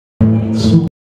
Manchi tu nell’aria/venerdì 1 marzo/Spazio Gloria di Como/MVI 4051
È iniziata la campagna Manchi tu nell’aria, una raccolta fondi per salvare lo Spazio Gloria di Como.Per lanciare l’iniziativa, venerdì 1 marzo è stato organizzato un incontro informativo, corredato da due interventi musicali dei 7grani, una festa dell’impegno gioioso per un bene comune. Per continuare a leggere vai sul sito